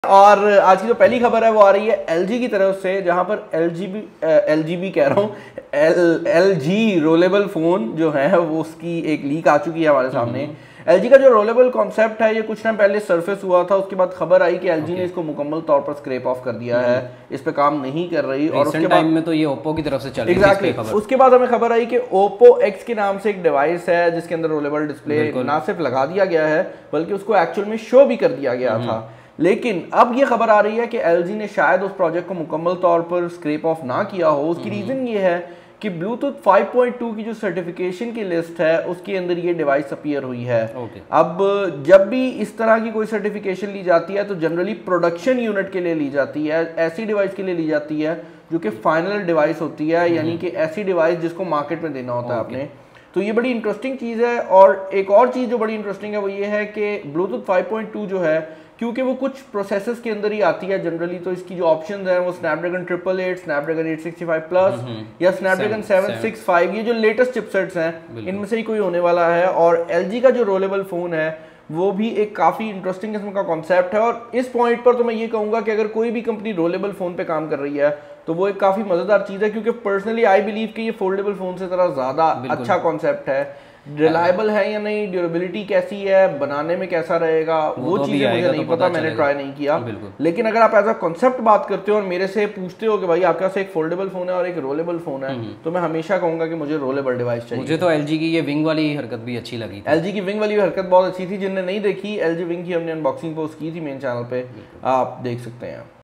LG is Going to Launch First Rollable Phone
The LG Rollable phone’s future has been clouded in doubt by recent reports and rumors that LG may sell its mobile device division. While LG has always maintained that the project is not dead, there’s now some concrete evidence to support its claims.